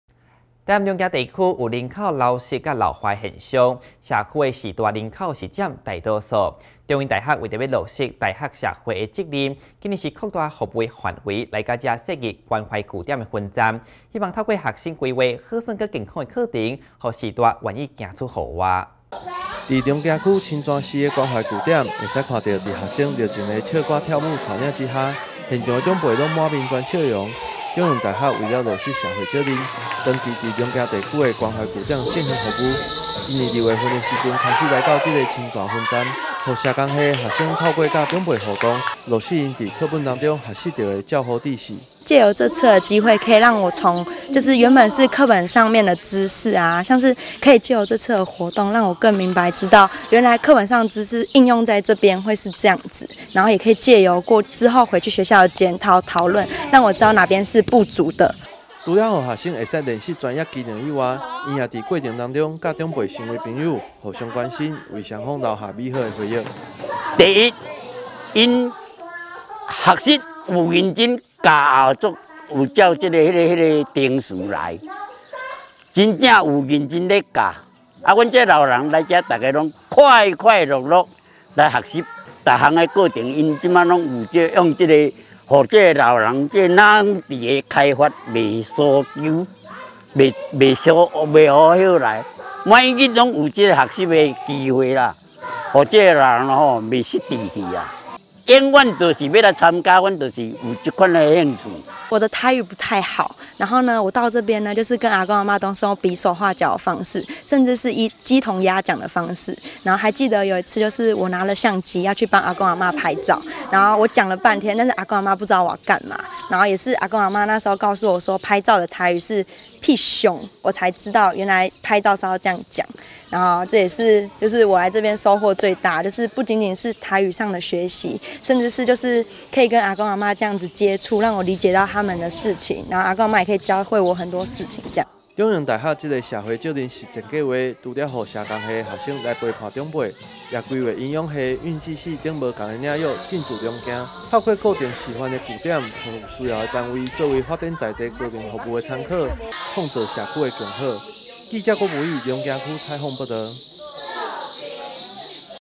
長大社工系深入偏鄉 鼓勵長者走出家門-新永安新聞
臺南龍崎地區有人口流失和老化的現象 社區的長輩人口佔了大多數 長榮大學為了落實 大學社會的責任 今年擴大服務範圍 來到這裡設立關懷據點的分站 希望透過學生規劃 好玩又健康的課程 讓長輩願意走出戶外